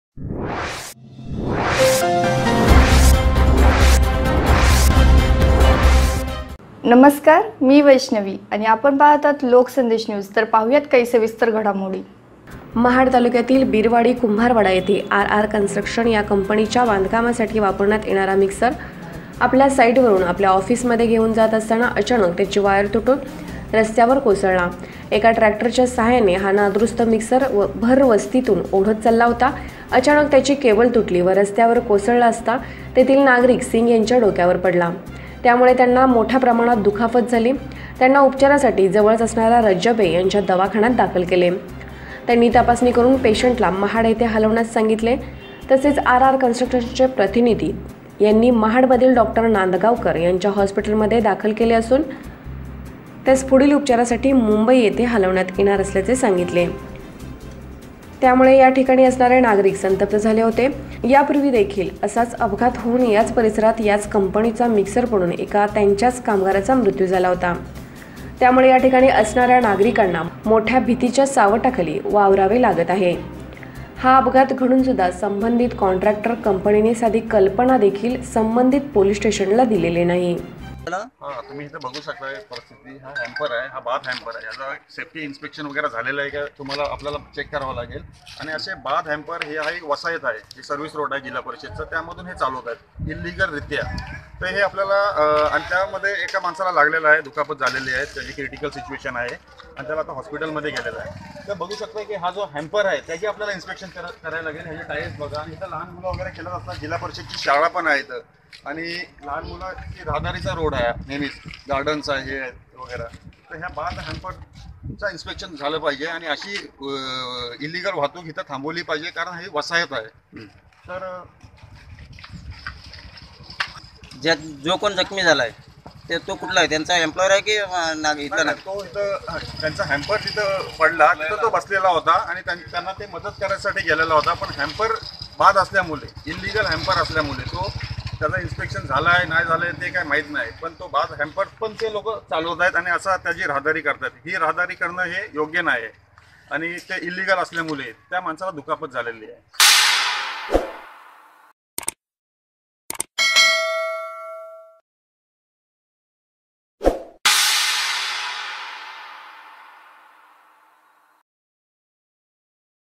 बिरवाडी कुंभारवाडा येथे अपघात एक इसम गंभीर जखमी i Raigad I
महाड तालुक्यातील बिरवाडी कुंभारवाडा येते आर आर कंट्रक्शन या कंपनीचा बांधकामासाठी वापरण्यात येणारा मिक्सर अापल्या साइटवरुन आपल्या हाॅफिस मध्ये घेऊन जात असताना अचानक त्याची वायर तुटून रस्त्यावर कोसळला येका टेक्टरच्या सायाने हा नादुरूस्त  मिक्स भर वस्तीतून वोडत चालला होता अचानक त्याची केबल तुटली व रस्त्यावर कोसळला असता तेथील नागरिक सिंग यांच्या डोक्यावर पडला त्यामुळे त्याला मोठ्याप्रमाणात दुखापत झाली त्याला उपचारासाठी जवळच असणाऱ्या रज्जबे यांच्या दवाखान्यात दाखल केले त्यांनी तपासणी करून पेसेंटला महाड हलवण्यास सांगितले म्हणून R. R कंट्रक्शनचे प्रतिनिधी त्यास महाड मधील डाॅ नांदगावकर यांच्या हाॅस्पिट्ल मध्ये दाखल केला असून त्यास पुढील उपचारासाठी मुंबईला हलवण्यात येणार असल्याचे सांगितले गेले आहे त्यामुळे याठिकाणी असणारे नागरिक संतप्त झाले होते यापूर्वी देखील असाच अपघात होऊन याच परिसरात याच कंपनीच्या मिक्सर पडून येका त्यांच्याच कामगाराचा म्रुत्यु झाला होता त्यामुळे याठिकाणी असणाऱ्या नागरिकांना मोठया भीतीच्या सावटा खाली वावरावे लागत आहे हा अपघात घडून सुद्धा संबंधित काँट्र्याकटर कंपनीने साधी कल्पना संबंधित पोलिस स्टेशनला दिला नाही